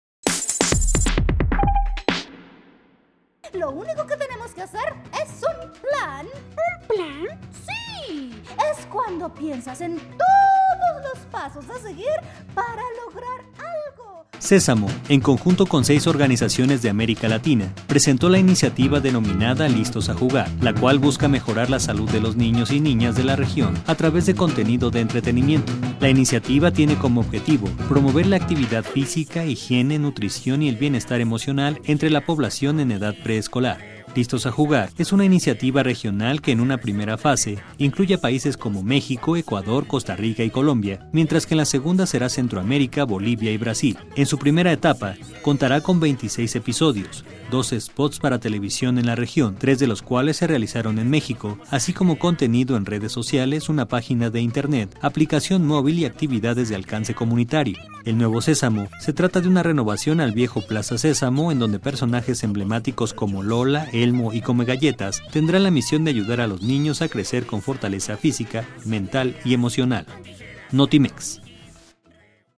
Sésamo presenta iniciativa para combatir obesidad infantil
En el proyecto participan Canal Once, las fundaciones Carlos Slim y Femsa, la Secretaría de Salud, UNICEF y Ecuador TV